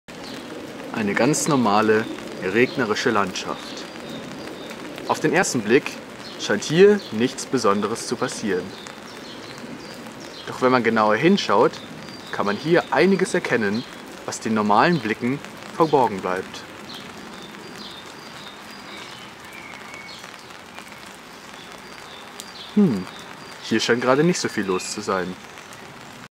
Umwerfende Aufnahmen von einem Goldkäfer! NATUR VOR DER HAUSTÜR
Hier ein paar spannende Aufnahmen von vor der Haustür. Da kann man auch schon mal einiges Entdecken. Also nicht zögern, selber rausgehen! ;) Wer den Blick riskiert kann sicher selber viel entdecken!
P.S. Sorry hab kein kein Makro objektiv. aber wer genau hinschaut sieht sicher den Käfer am rechten unteren Bildrand!

P.P.S. Vielen Dank an Leonhardt für das wirklich tolle Thumpnail Foto! Quelle